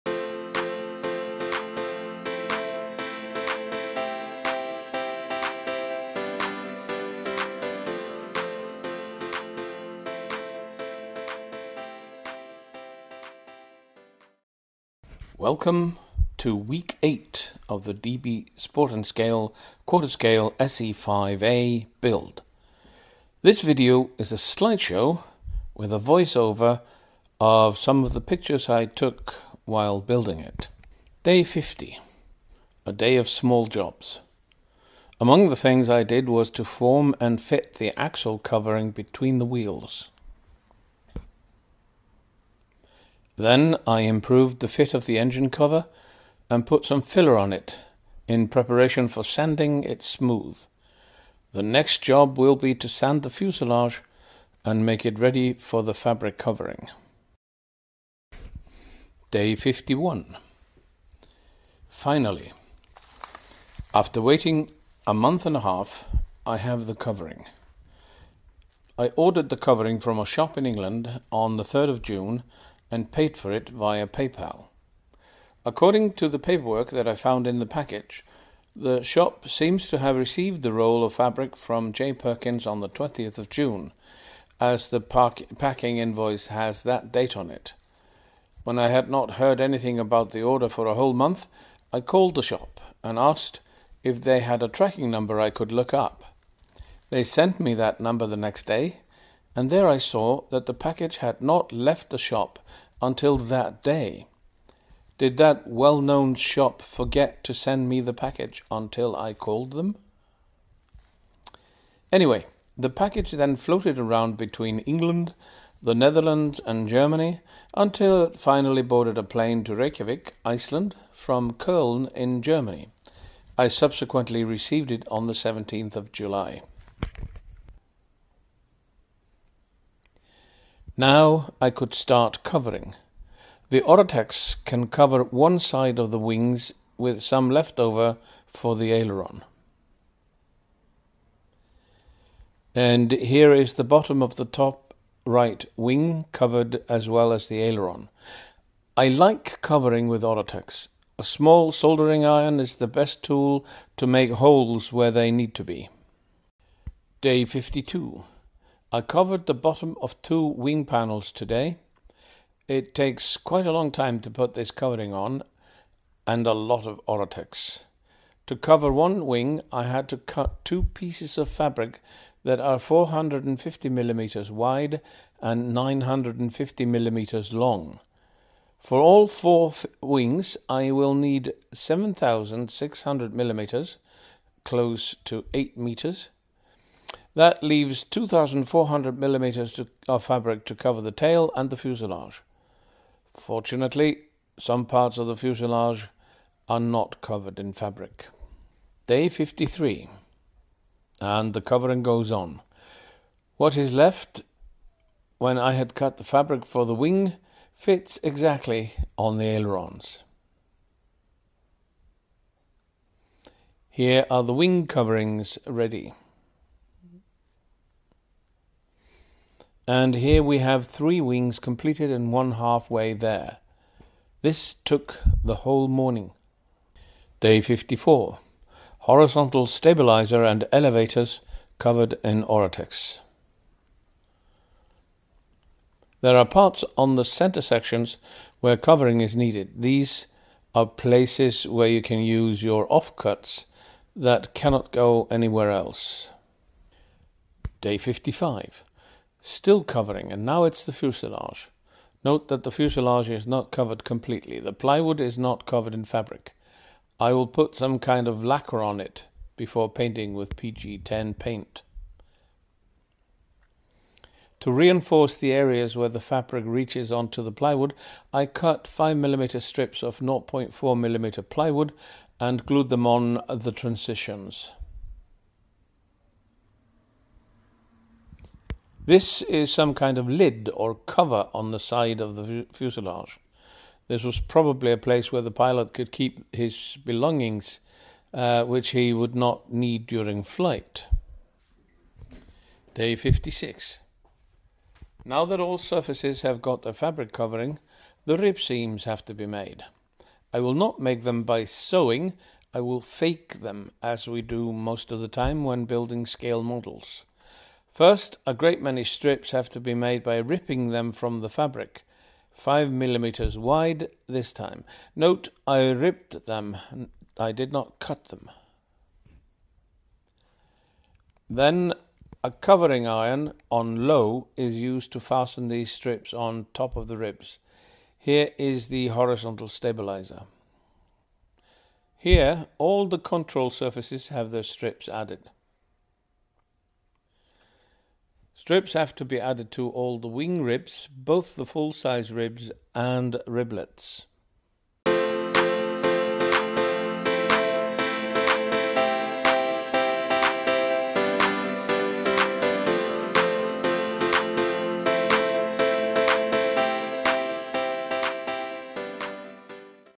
Building the S.E.5a: week 8
Slide show of me building the S.E.5a.